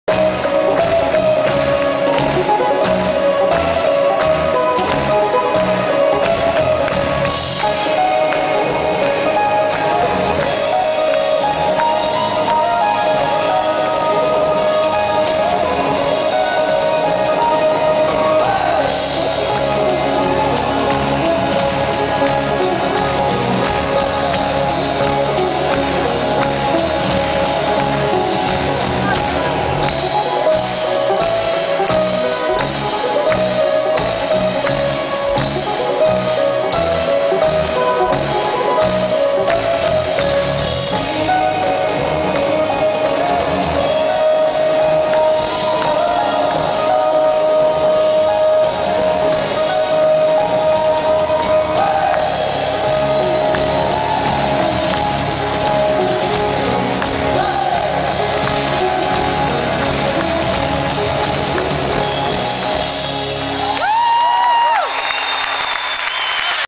Sons of Korah lied 2 Katwijk
Sons of Korah spelen in de nieuwe kerk in Katwijk